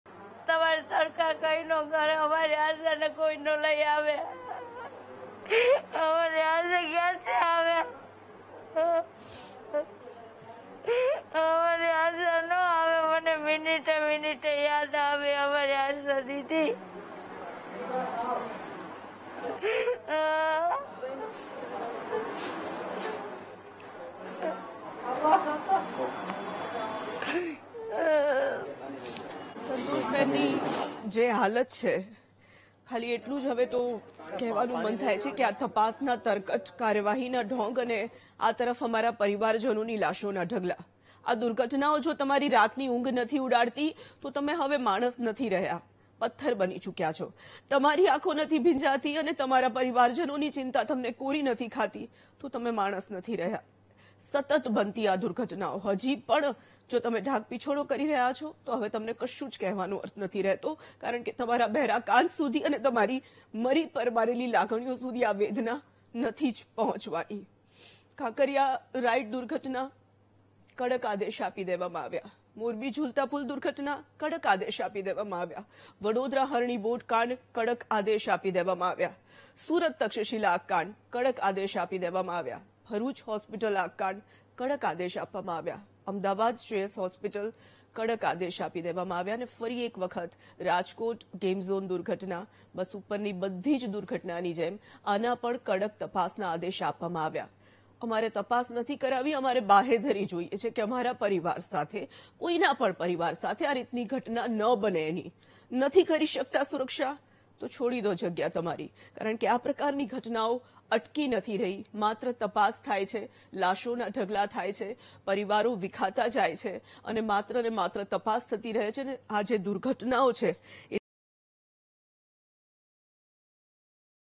Rajkot Tregady: આશા કાથડની બહેન ધ્રુસકે ને ધ્રુસકે રડી પડી, જોનારાની આંખમાં આસું | VTV Gujarati
VIDEO: આશા કાથડની બહેન ધ્રુસકે ને ધ્રુસકે રડી પડી, જોનારાની આંખમાં આસું